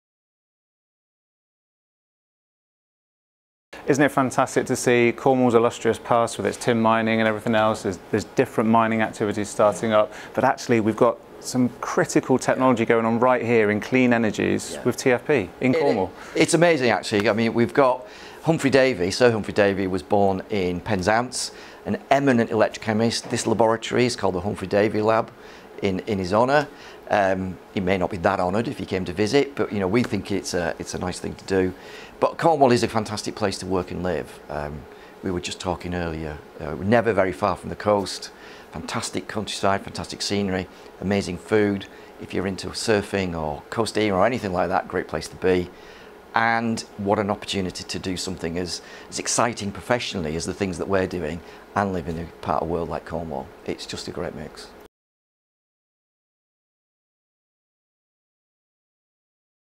Next Generation Hydrogen Technology: Made in Cornwall
David Hodgson, Managing Director at TFP Hydrogen Products, discusses  hydrogen technology being produced in Cornwall and its ties with regions past.